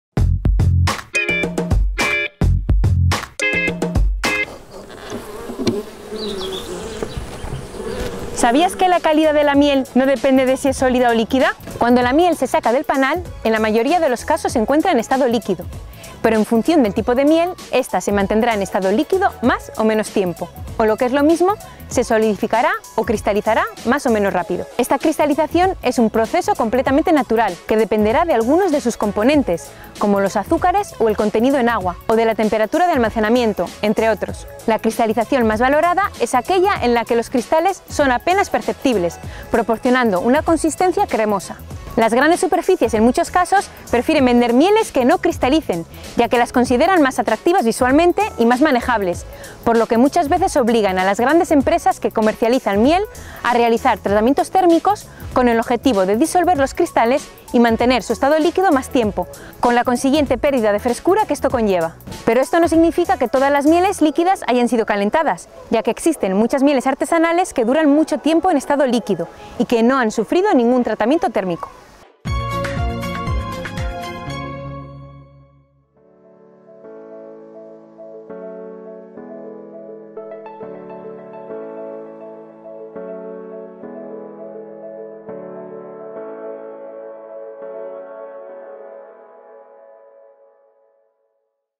¿SabíasQue...? La calidad de la miel
¿Te apasiona la nutrición? ¡Infórmate sobre el Grado en Nutrición Humana y Dietética de la Universidad Isabel I!
¿Sabías que la calidad de la miel no depende de si es sólida o líquida?

Ana Pascual Coordinadora del Grado en Nutrición Humana y Dietética nos cuenta que cuando la miel se saca del panal, en la mayoría de los casos se encuentra en estado líquido. Pero en función del tipo de miel, esta se mantendrá en estado líquido más o menos tiempo, o lo que es lo mismo, se solidificará o cristalizará más o menos rápido.

Las grandes superficies en muchos casos prefieren vender mieles que no cristalicen, ya que las consideran más atractivas visualmente y más manejables, por lo que muchas veces obligan a las grandes empresas que comercializan miel a realizar tratamientos térmicos con el objetivo de disolver los cristales y mantener su estado líquido más tiempo, con la consiguiente pérdida de frescura que esto conlleva.

Pero esto no significa que todas las mieles líquidas hayan sido calentadas, ya que existen muchas mieles artesanales que duran mucho tiempo en estado líquido y que nunca han sufrido ningún tratamiento térmico.